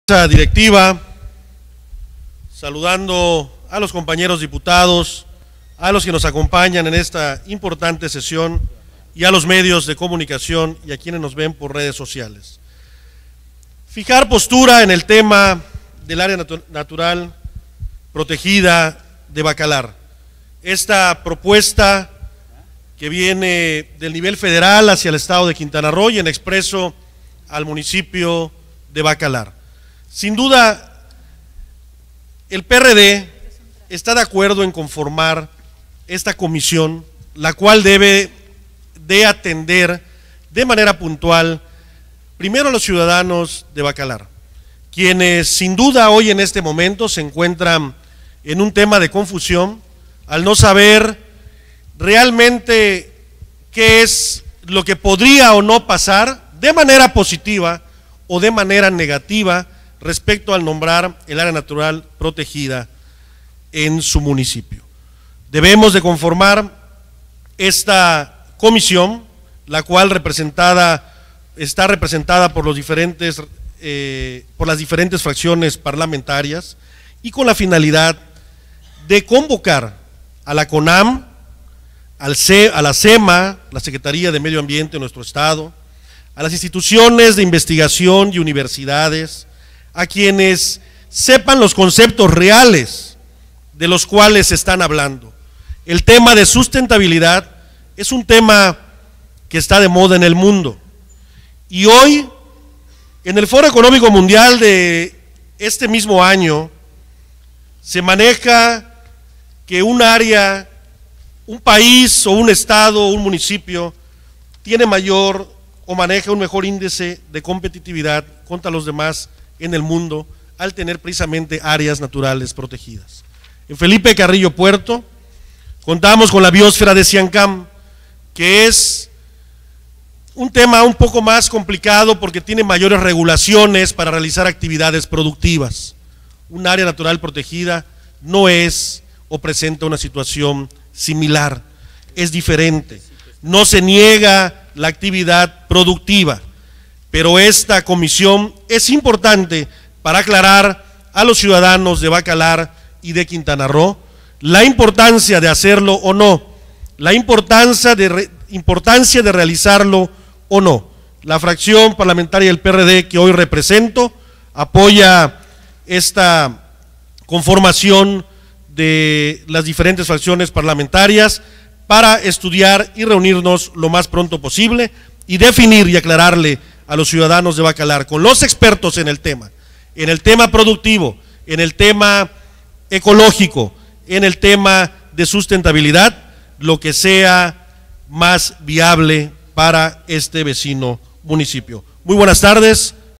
Intervención del Dip. José Esquivel Vargas. PRD (23/Oct/2017
Intervención del Dip. José Esquivel Vargas en relación a la lectura del Acuerdo de urgente y obvia resolución por el que se exhorta a la Gran Comisión de la Honorable XV Legislatura del Congreso del Estado de Quintana Roo, para que determine la instalación de una Comisión Especial de Investigación y Análisis sobre el impacto de la Creación del Área Natural Protegida en el Sistema Lagunar de Bacalar; Presentado por los Diputados Eduardo Lorenzo Martínez Arcila, Mayuli Latifa Martínez Simón, Fernando Levin Zelaya Espinoza, Eugenia Guadalupe Solís Salazar, Gabriela Angulo Sauri y Jesús Alberto Zetina Tejero, todos integrantes del grupo parlamentario del Partido Acción Nacional; José Esquivel Vargas, Laura Esther Beristain Navarrete y Emiliano Vladimir Ramos Hernández, integrantes del Grupo Parlamentario del Partido de la Revolución Democrática; Ramón Javier Padilla Balam, Coordinador del Grupo Parlamentario del Partido Nueva Alianza; Carlos Mario Villanueva Tenorio, Coordinador del Grupo Parlamentario del Partido Encuentro Social; y Juan Ortiz Vallejo, Diputado Independiente; para su aprobación en su caso.